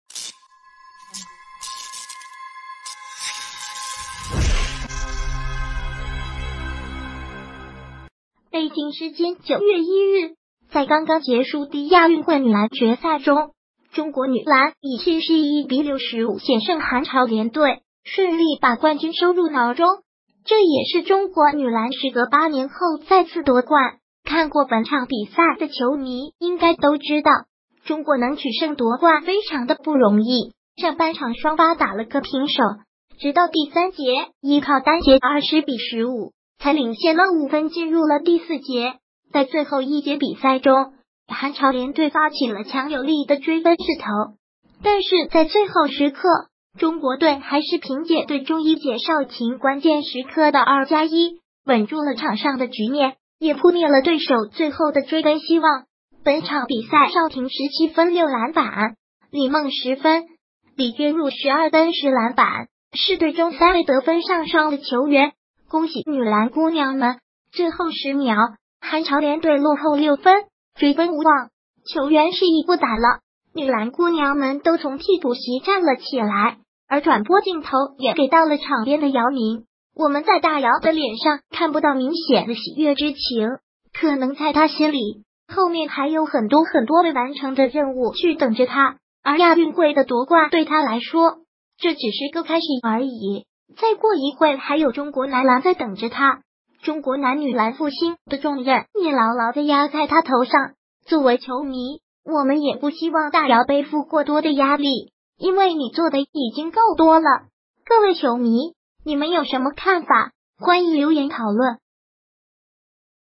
最后10秒 韩朝联队示意不打了 镜头扫过 有谁留意到大姚的表情
北京时间9月1日，在刚刚结束的亚运会女篮决赛中，中国女篮以71-65险胜韩朝联队，顺利把冠军收入囊中，这也是中国女篮时隔8年后再次夺冠。看过本场比赛的球迷应该都知道，中国能取胜夺冠非常的不容易，上半场双发打了个平手，直到第三节依靠单节20-15才领先了5分进入了第四节，在最后一节比赛中，韩朝联队发起了强有力的追分势头，但是在最后时刻，中国队还是凭借队中一姐邵婷关键时刻的2+1，稳住了场上的局面，也扑灭了对手最后的追分希望。本场比赛邵婷17分6篮板，李梦10分，李月汝12分10篮板，是队中3位得分上双的球员，恭喜女篮姑娘们。最后10秒，韩朝联队落后6分，追分无望，球员示意不打了。女篮姑娘们都从替补席站了起来，而转播镜头也给到了场边的姚明，我们在大姚的脸上看不到明显的喜悦之情，可能在他心里，后面还有很多很多未完成的任务去等着他，而亚运会的夺冠对他来说，这只是个开始而已，再过一会还有中国男篮在等着他。中国男女篮复兴的重任也牢牢的压在他头上。作为球迷，我们也不希望大姚背负过多的压力，因为你做的已经够多了，各位球迷，你们有什么看法，欢迎留言讨论。